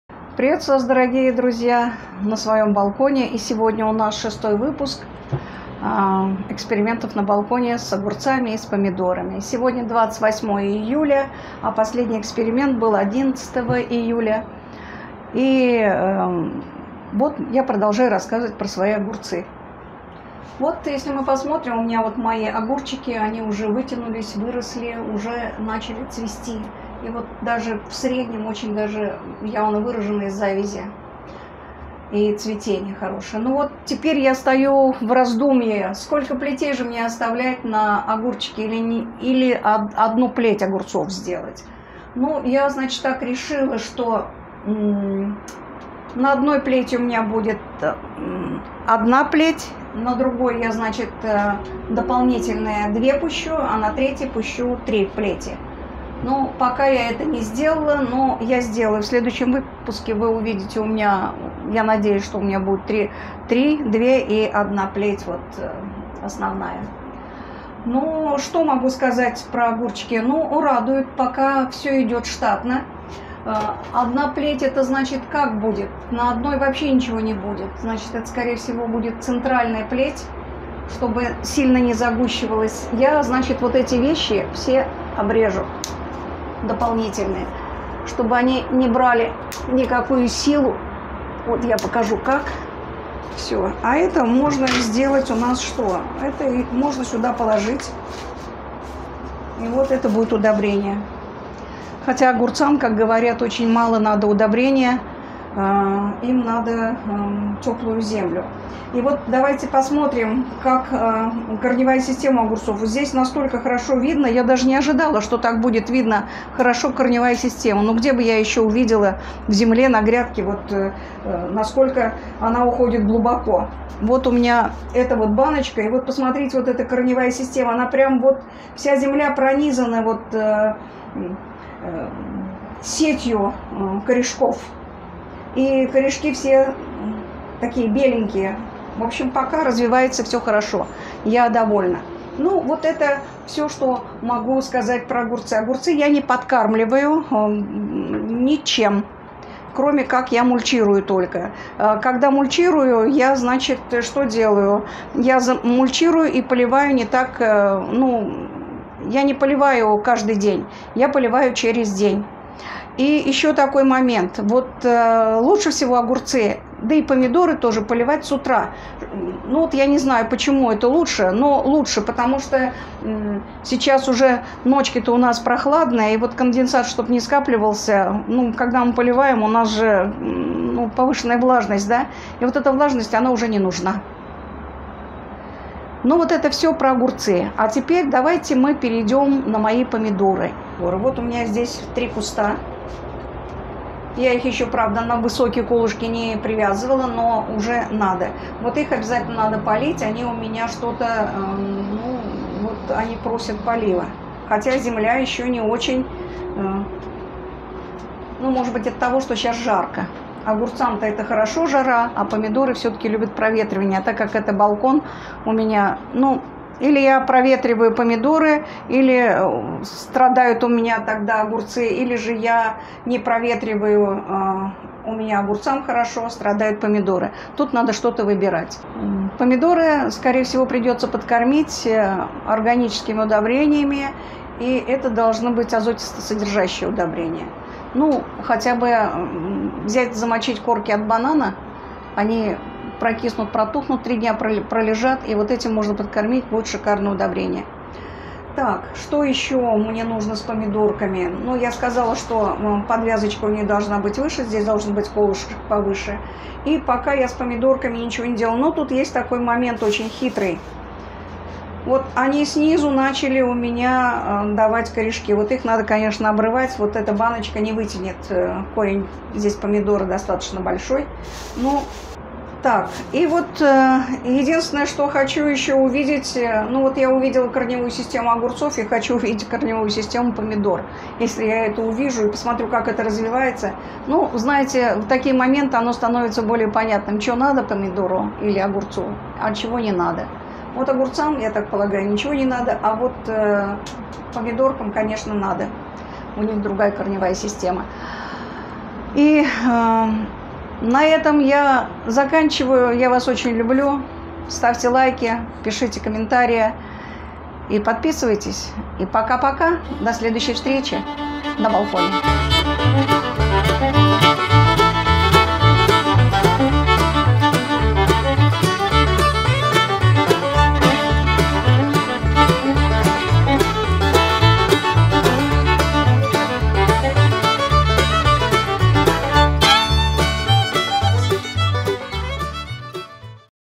🍅🥒Выращиваем овощи огурцы и помидоры на балконе 🍅🥒 Эксперимент - #6
6-й выпуск с отчетом о результатах эксперимента по выращиванию овощей огурцов и помидоров на балконе или подоконнике в городских условиях. Конец июля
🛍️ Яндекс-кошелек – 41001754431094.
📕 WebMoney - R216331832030
😎 Карта-Сбербанк - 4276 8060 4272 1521